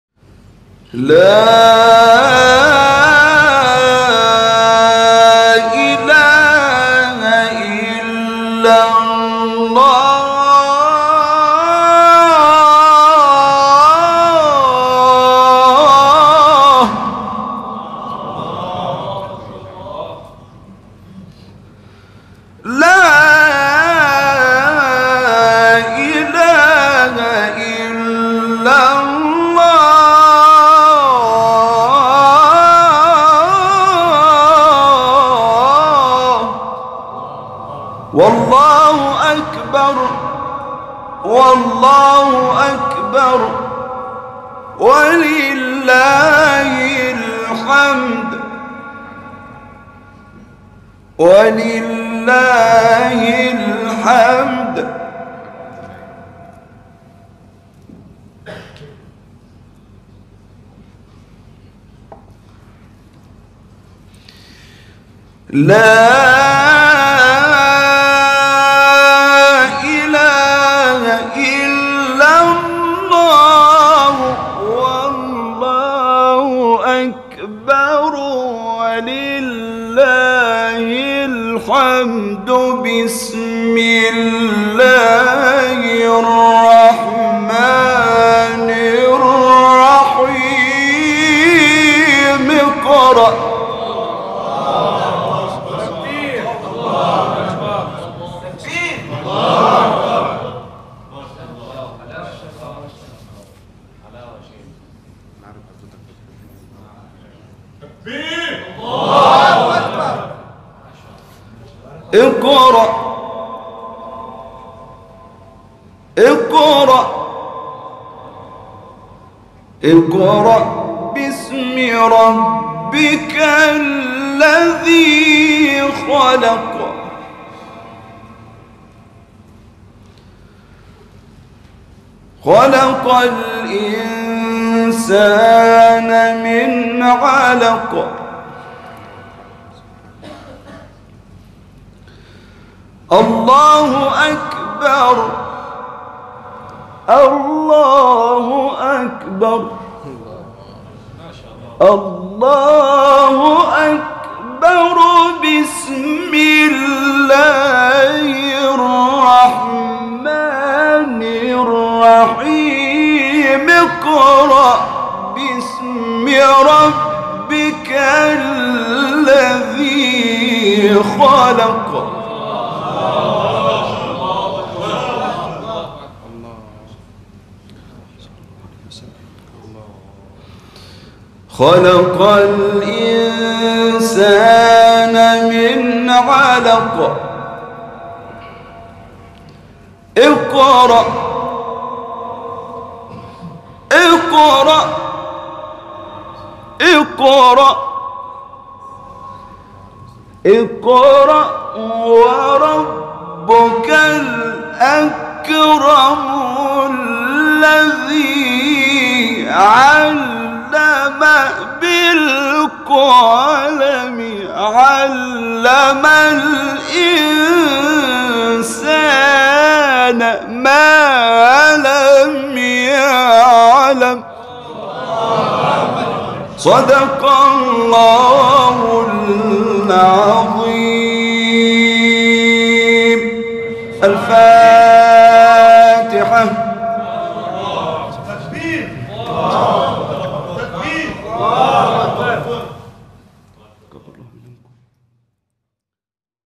العاصمه مابوتو 😲 موزمبيق 🕌تلاوة تاريخيه للشيخ د عبدالناصرحرك 👍 Mozambique Maputo Harak TV
Qari Dr Abdelnasser is reciting amazing Recitation at Maputo.  Mozambique 👍👍👍👍👍